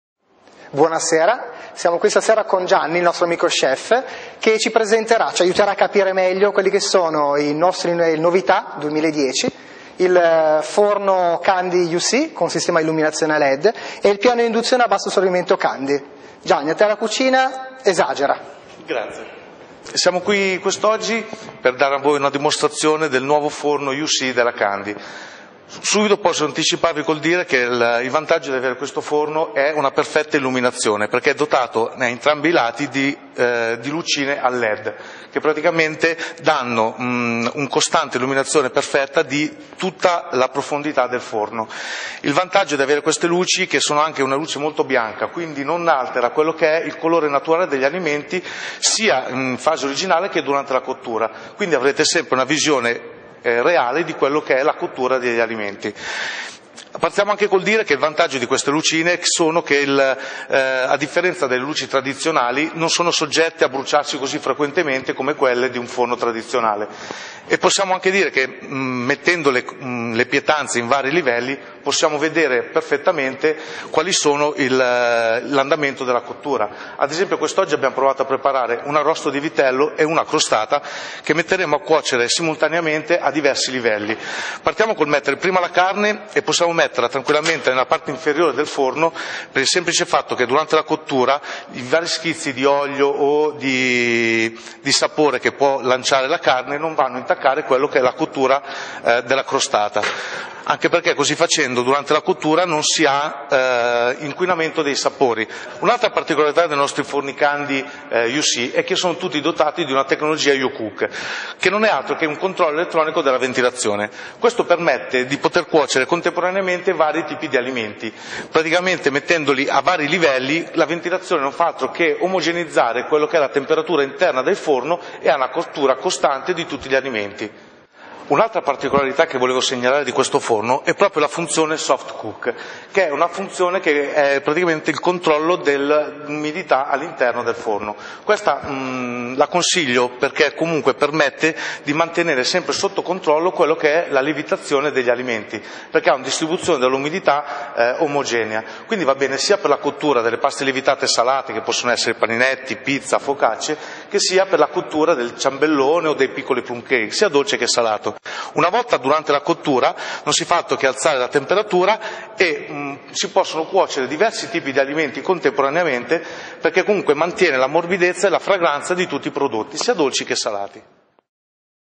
Candy Forno con illuminazione a led U-See.flv
Scuola di cucina con il nuovo forno candy con illuminazione a led U-See visione perfetta della cotura degli alimenti